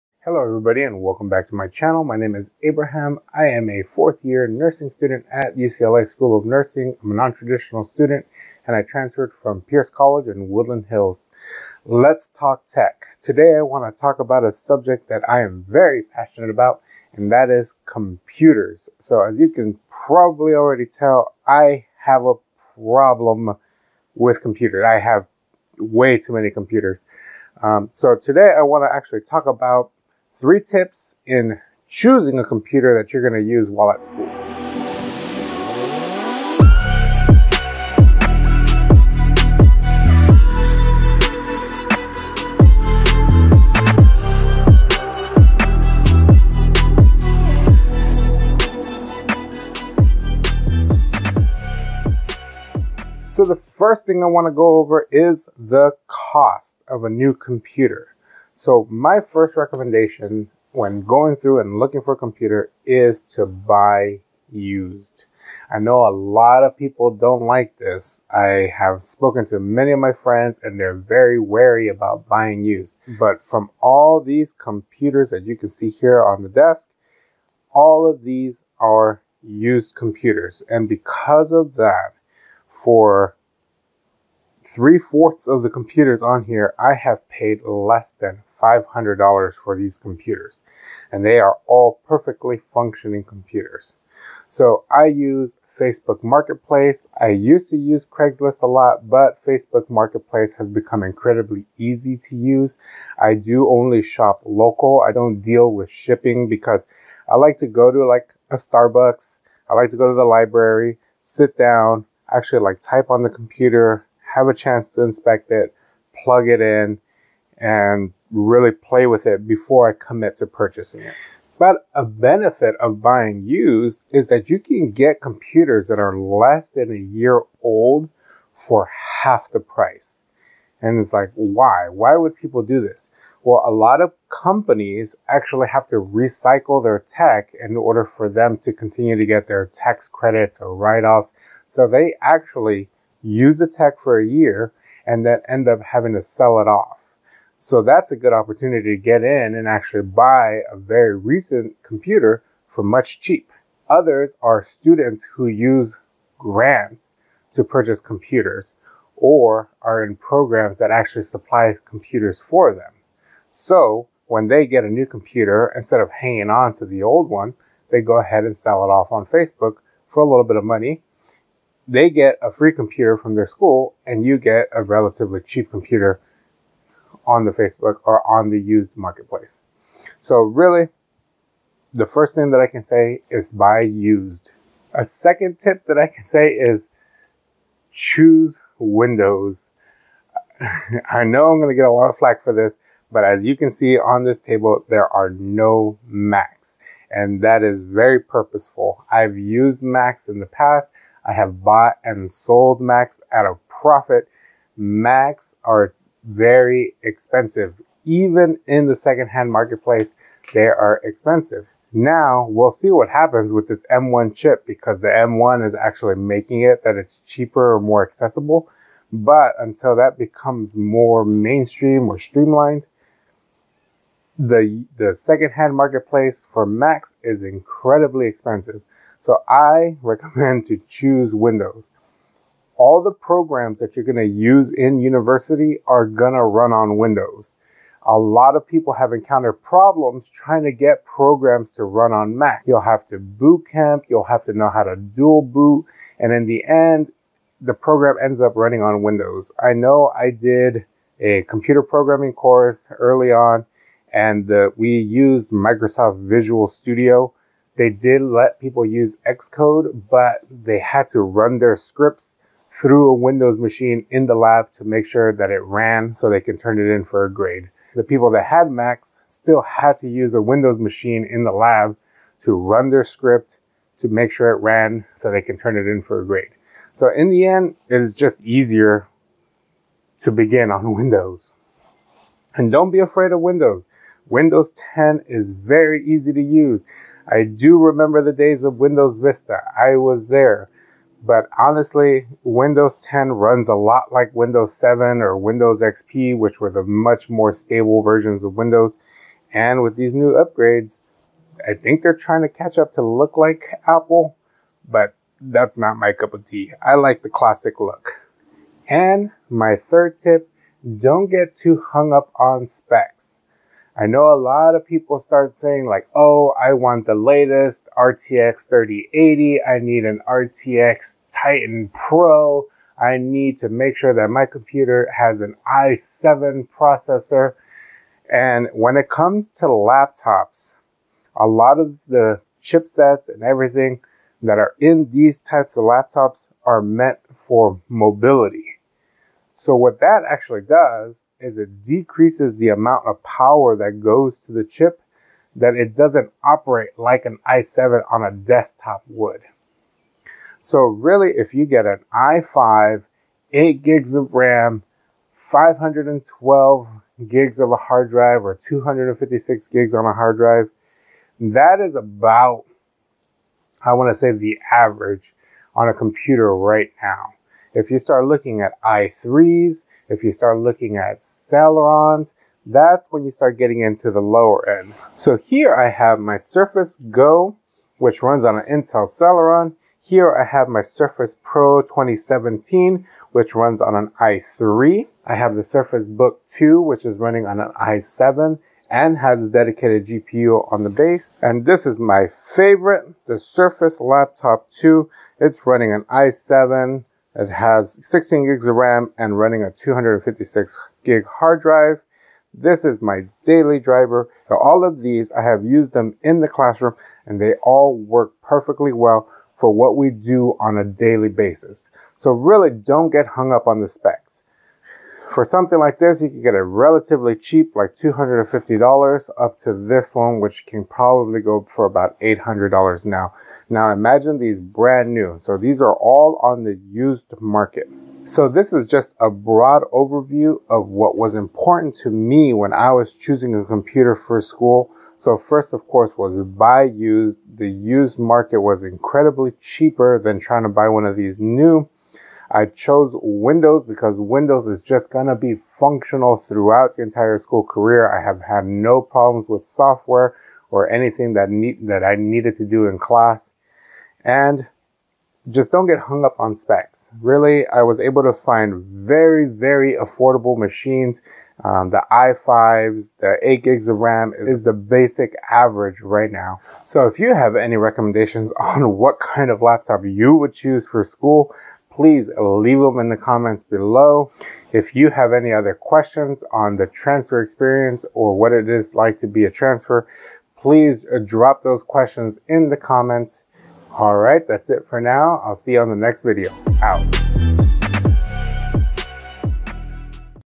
I Have an Unhealthy Relationship with Technology! Looking at Laptops for School!
Hello, Everyone, My Name is Abraham and I am a 4th year nursing student, transfer student, and non-trad. Today I look at what laptops I used in college and simple tips of what to look for when buying a laptop for school!
Camera: Sony HDR-CX900
Wireless Mic/Transmitter: Sony ECMAW4